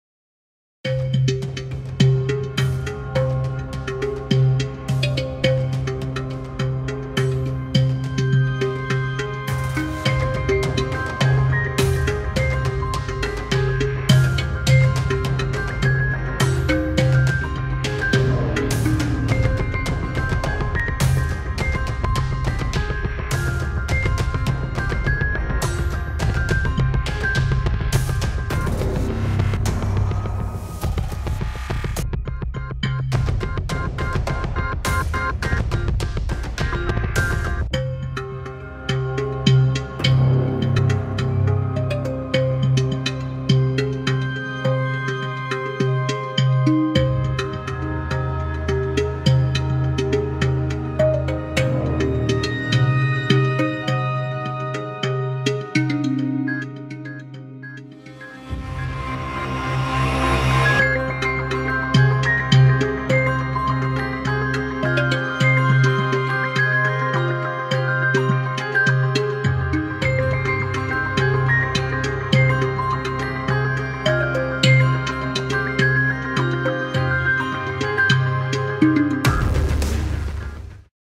Red-Handed AWARD-WINNING, INTERNATIONALLY ACCLAIMED one-act play for schools and drama societies
This award-winning, internationally acclaimed one-act play is about a group of boys who try to steal exam papers.